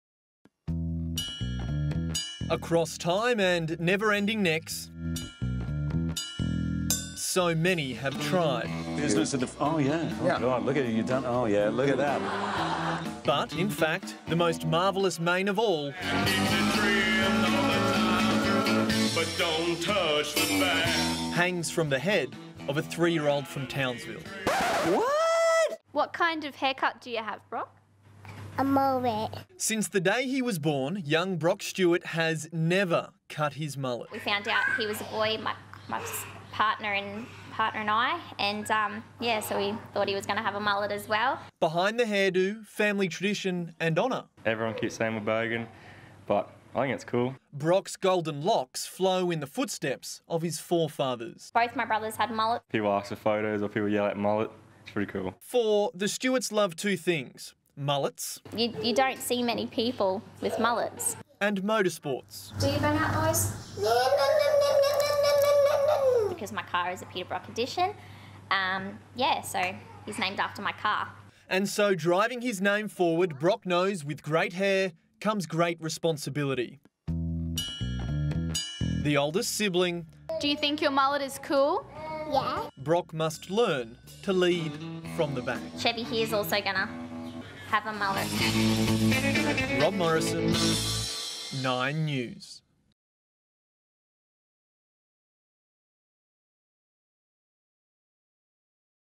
A Townsville 3-year-old is giving the mullet a comeback | Nine News Australia
Brock Weight, a 3-year-old from Cranbrook, Townsville, loves rocking the mullet which runs in his family.